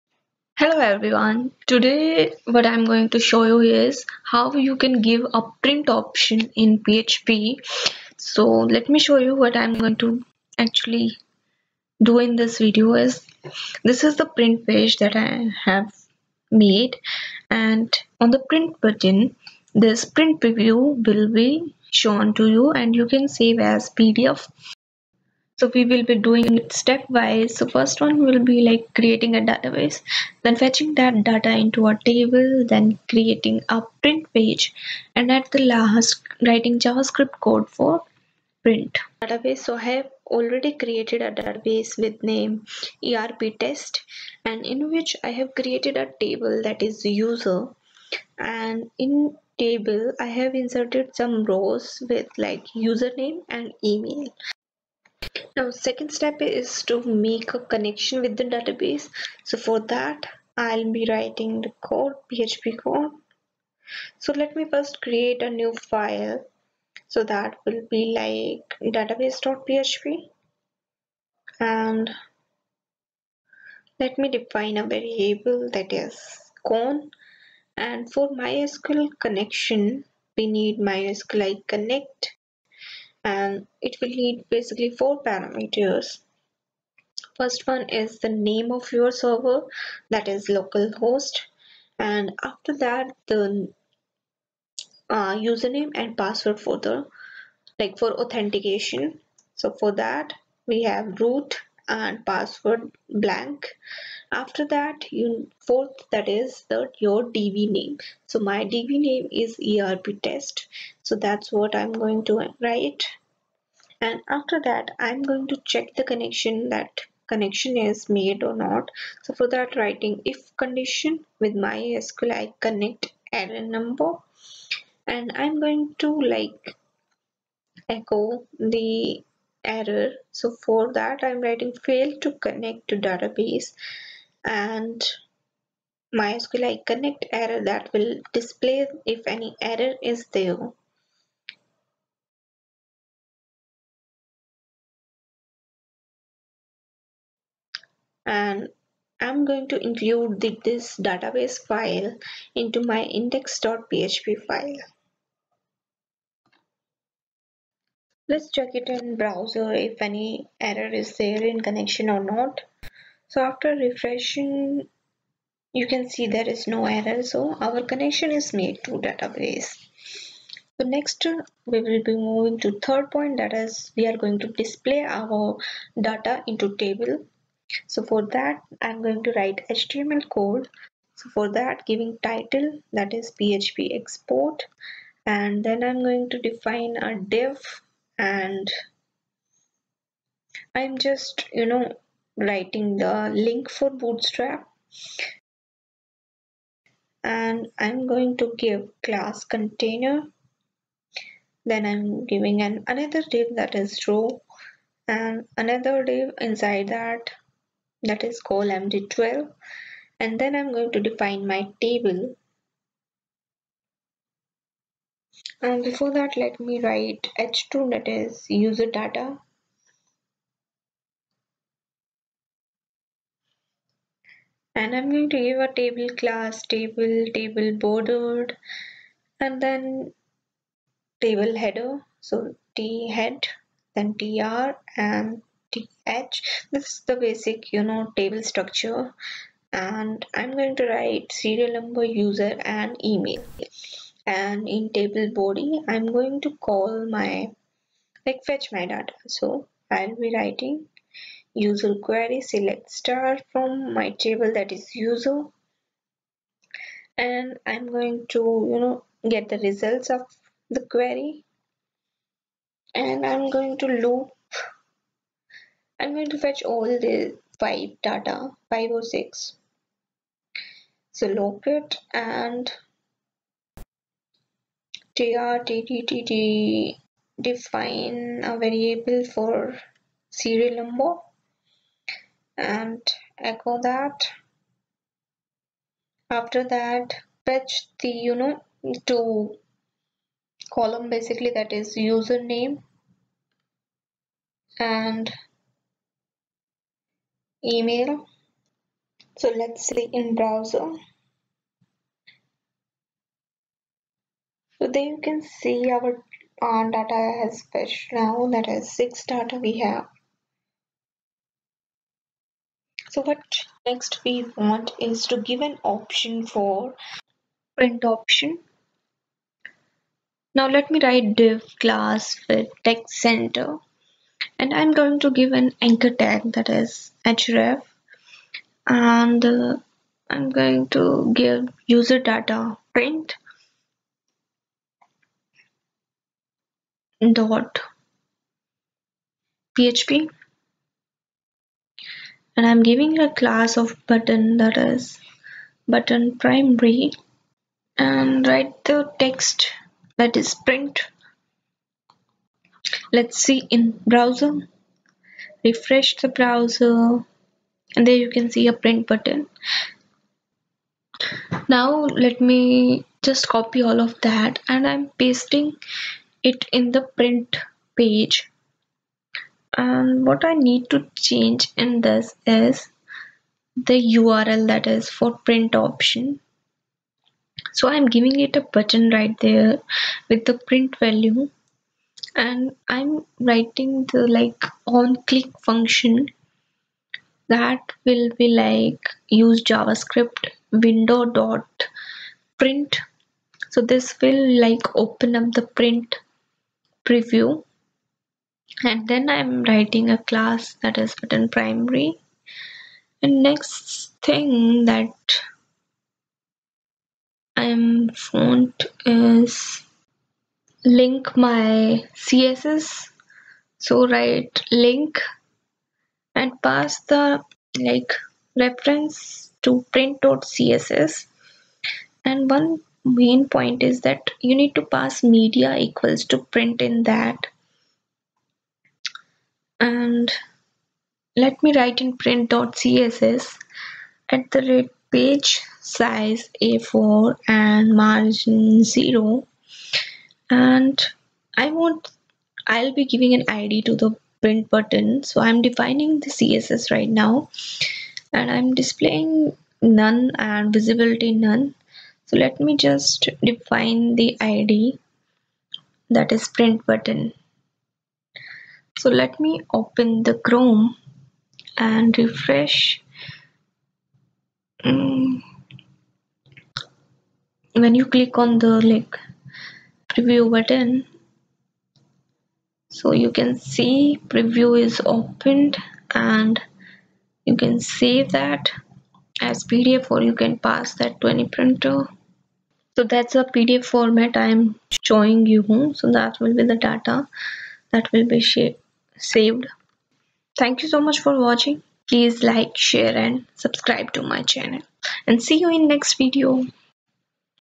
Print Option in PHP using Javascript | Save in Pdf | PHP Beginner Tutorial
Print Option in PHP | Save in Pdf | PHP Beginner Tutorial

This video tutorial cover how you can save the web page in pdf and take print of the web page.

Print option is basically needed where you have to work with invoice, delivery order and many more.

 Steps:
Create a database
Make a connection with database
fetch the data in web page
Give print option - save as pdf or print

Javascript function used: window.print() for print preview

Css for removing the print button in print preview. Media attribute of link with value Print is used for applying css on print preview